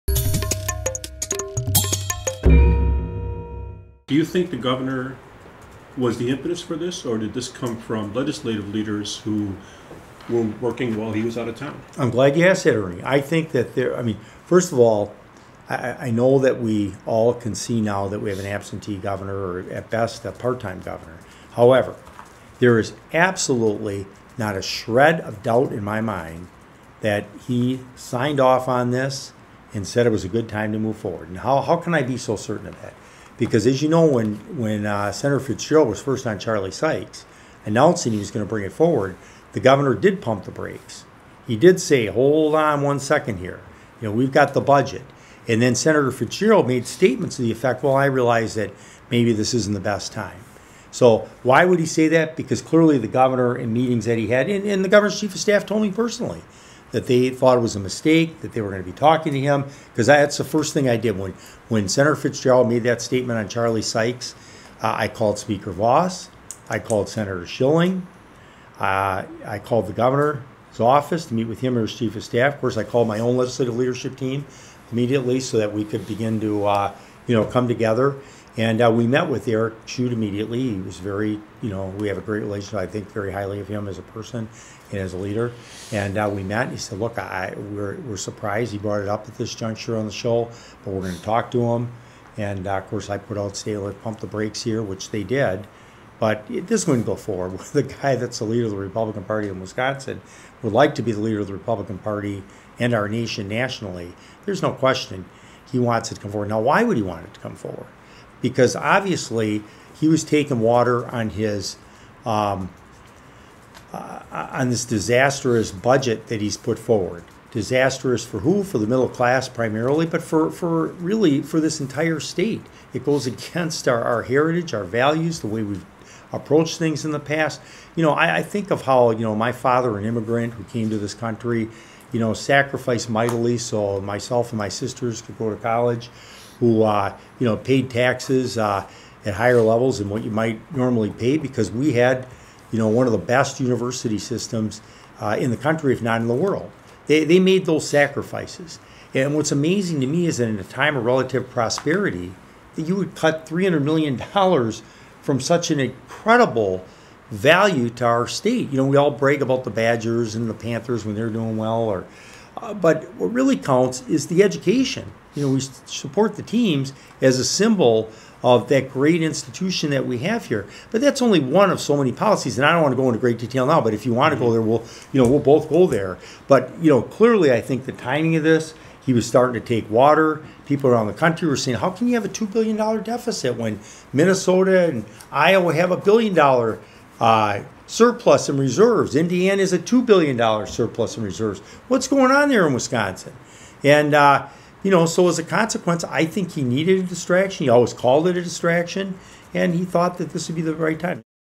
Peter Barca on right to work
In a discussion with Journal Sentinel opinion writers, editors and reporters, Assembly Minority Leader Peter Barca discusses Gov. Scott Walker and right-to-work legislation.